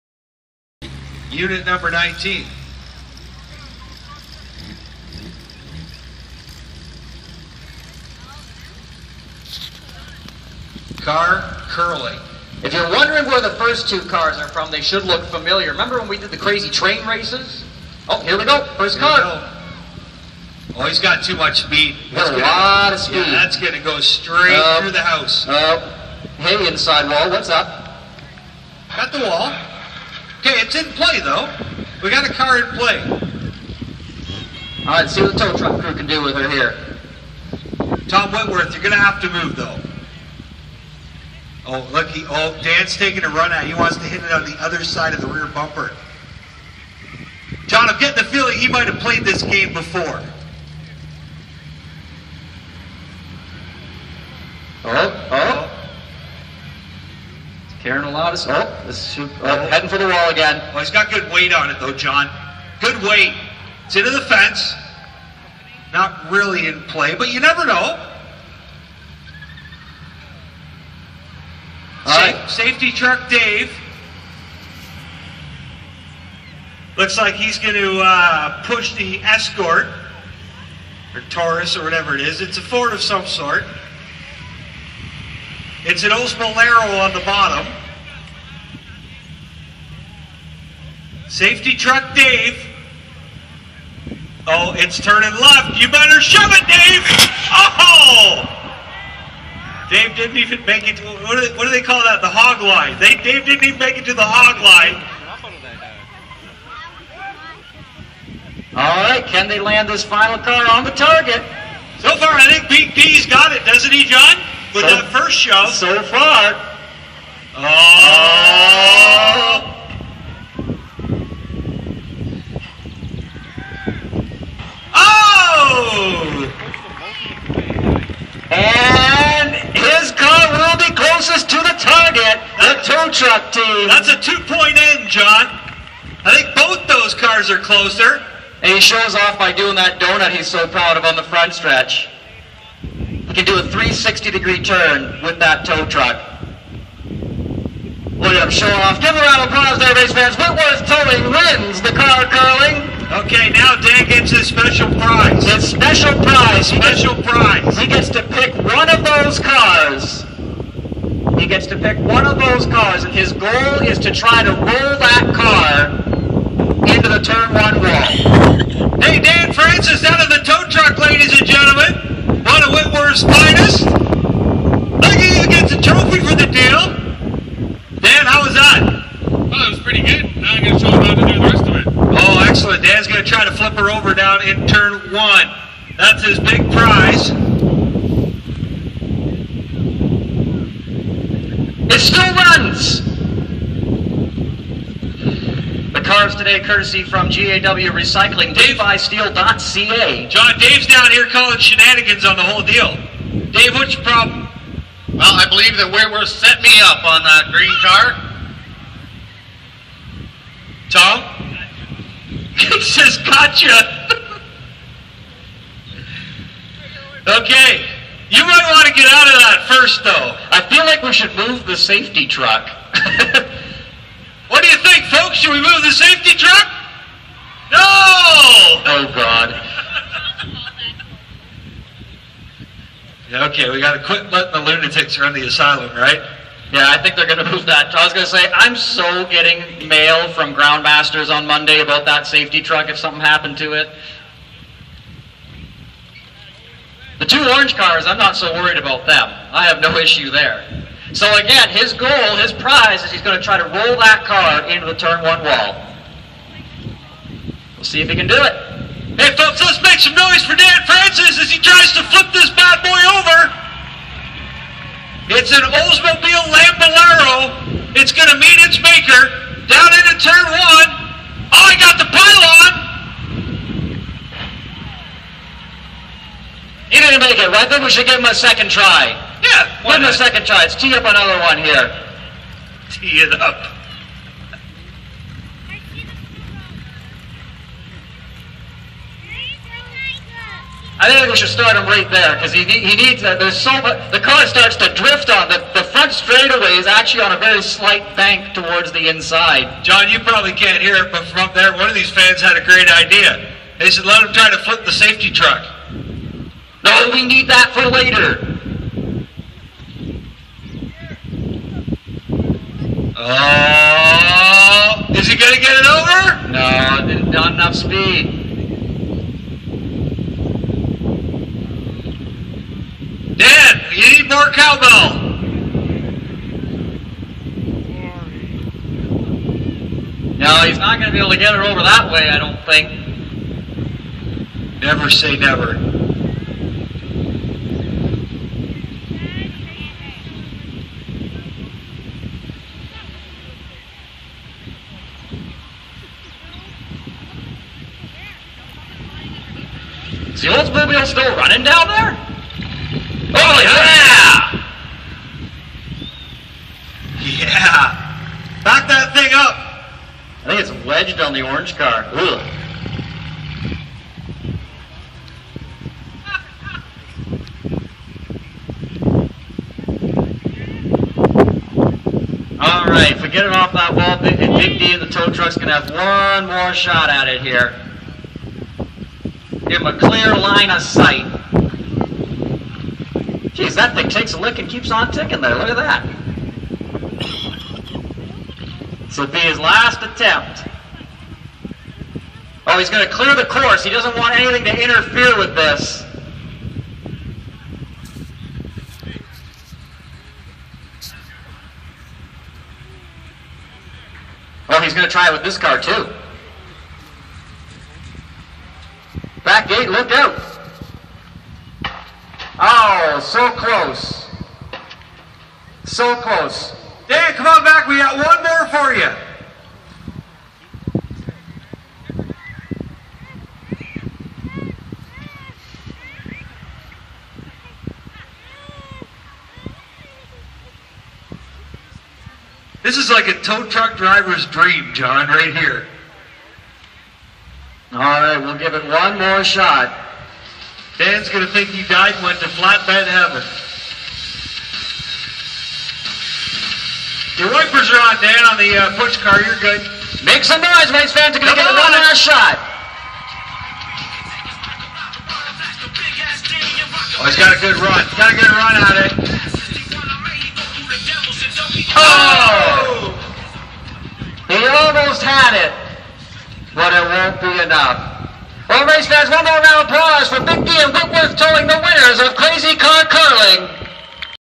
Crazy car curling at delaware speedway
Car curling between the saftey crew and tow truck drivers